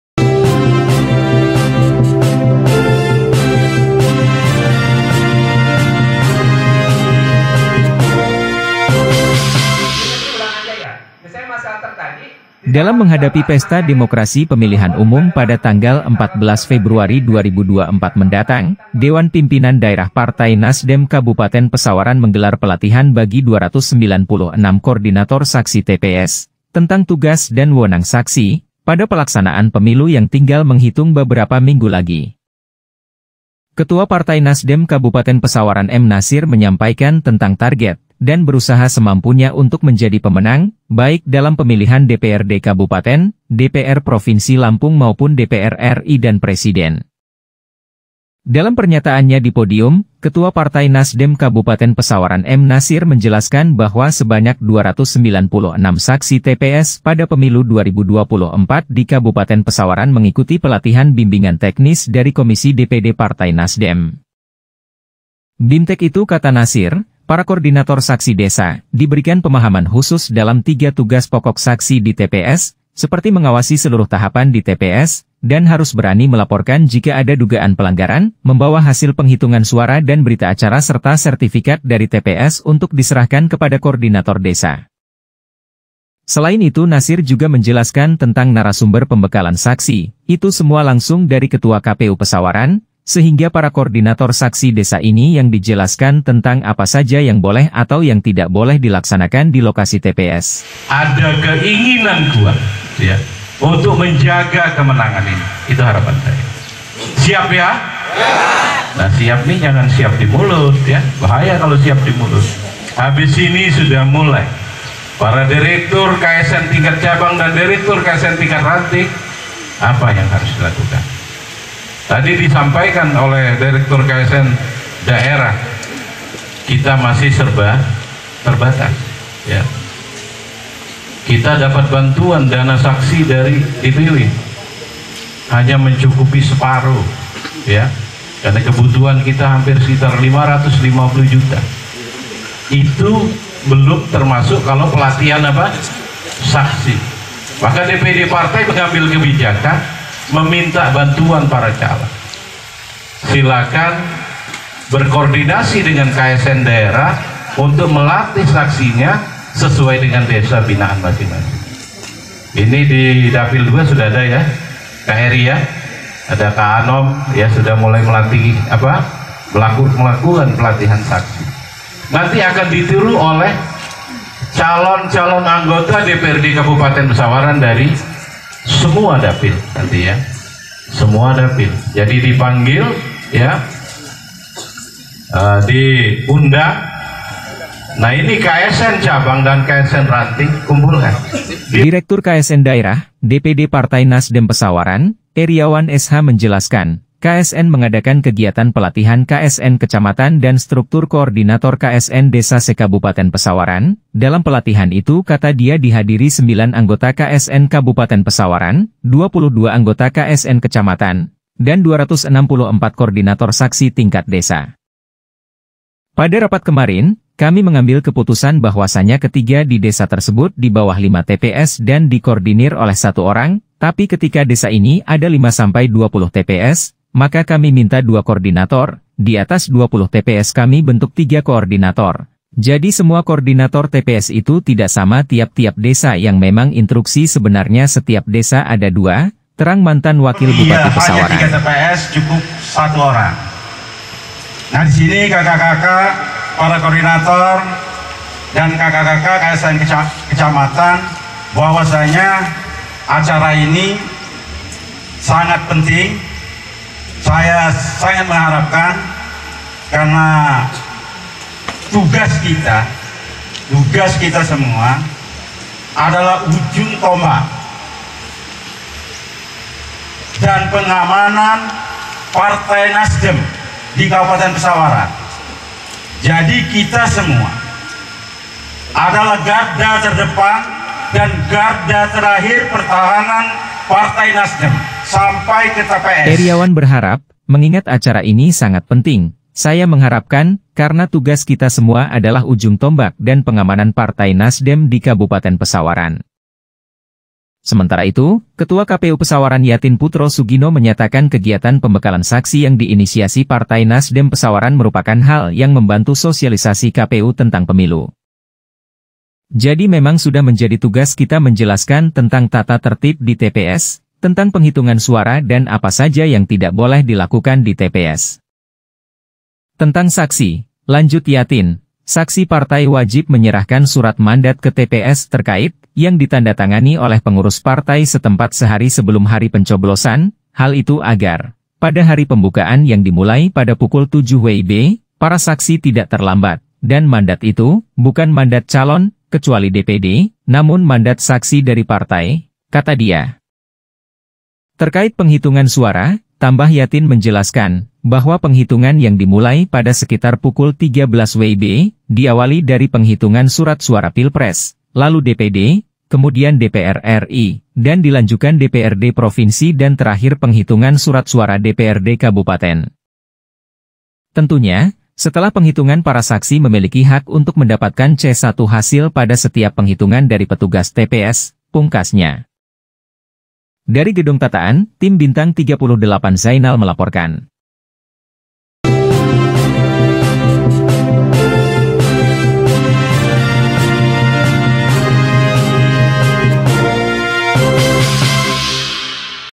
296 Kordinator Saksi TPS Partai Nasdem Kabupaten Pesawaran Ikuti Pelatihan.
Dalam Menghadapi pesta demokrasi Pemilihan umum pada tanggal 14 Februari 2024 mendatang, Dewan Pimpinan Daerah Partai Nasdem Kabupaten Pesawaran menggelar Pelatihan bagi 296 kordinator saksi TPS , tentang tugas dan wewenang saksi, pada pelaksanaan pemilu yang tinggal menghitung baberapa minggu lagi.

Ketua Partai Nasdem kabupaten Pesawaran M Nasir menyampaikan tentang target, dan berusaha semampu nya untuk menjadi pemenang, baik dalam pemilihan DPRD kabupaten, DPR Provinsi lampung maupun DPR R I dan Presiden.

Dalam pernyataan nya dipodiyum, ketua Partai Nasdem Kabupaten Pesawaran M Nasir menjelaskan bahwa Sebanyak 296 saksi TPS pada Pemilu 2024 di Kabupaten Pesawaran  mengikuti pelatihan bimbingan teknis dari Komisi DPD Partai Nasdem.

Bimtek itu kata Nasir, para kordinator saksi desa, diberikan pemahaman khusus dalam 3 tugas pokok saksi di TPS, seperti mengawasi seluruh tahapan di TPS, dan harus berani melaporkan jika ada dugaan pelanggaran, membawa hasil penghitungan suara dan berita acara serta sertifikat dari TPS untuk diserahkan kepada kordinator desa.

selain itu Nasir juga menjelaskan tentang Narasumber pembekalan saksi, itu semua langsung dari Ketua KPU Pesawaran, sehingga para kordinator saksi desa ini yang dijelaskan tentang apa saja yang boleh atau yang tidak boleh dilaksanakan di lokasi TPS,"

Direktur KSN Daerah, DPD Partai Nasdem Pesawaran, Eriawan SH menjelaskan, KSN mengadakan kegiatan pelatihan KSN Kecamatan dan struktur koordinator KSN Desa se-Kabupaten Pesawaran, dalam pelatihan itu kata dia Dihadiri 9 anggota KSN Kabupaten Pesawaran, 22 anggota KSN Kecamatan, dan 264 kordinator saksi tingkat desa.

"Pada rapat kemarin, kami mengambil keputusan bahwasanya ketiga di desa tersebut dibawah 5 TPS dan di kordinir oleh 1 orang, Tapi ketika desa ini ada 5 sampai 20 TPS, maka kami minta 2 kordinator, diatas 20 TPS kami bentuk 3 koordinator,  jadi semua kordinator TPS itu tidak sama tiap tiap desa yang memang intruksi sebenarnya setiap desa ada 2,"terang mantan Wakil Bupati Pesawaran.

Eriawan berharap, mengingat acara ini sangat penting, saya mengharapkan,  karna tugas kita semua adalah ujung tombak dan pengamanan Partai NasDem di Kabupaten Pesawaran.

Sementara itu, Ketua KPU Pesawaran Yatin Putro Sugino menyatakan kegiatan pembekalan saksi yang diinisiasi Partai NasDem Pesawaran merupakan hal yang membantu sosialisasi KPU tentang pemilu.

"Jadi memang sudah menjadi tugas kita menjelaskan tentang tata tertib di TPS, tentang penghitungan suara dan apa saja yang tidak boleh dilakukan di TPS."

Tentang Saksi, lanjut Yatin, Saksi Partai wajib menyerahkan surat mandat ke TPS terkait, yang ditandatangani oleh pengurus partai setempat sehari sebelum hari pencoblosan, Hal itu agar, pada hari pembukaan yang dimulai pada pukul 7.00 WIB, para saksi tidak terlambat, dan mandat itu, bukan mandat calon (kecuali DPD), namun mandat saksi dari partai," Kata dia.

Terkait penghitungan suara, tambah Yatin menjelaskan, bahwa penghitungan yang dimulai pada sekitar pukul 13.00 WIB, diawali dari penghitungan surat suara Pilpres, lalu DPD, kemudian DPR R I, dan dilanjukan DPRD Provinsi dan terakhir penghitungan surat suara DPRD Kabupaten.

"Tentunya, setelah penghitungan para saksi memiliki hak untuk mendapatkan C1 Hasil pada setiap penghitungan dari petugas TPS,"pungkasnya.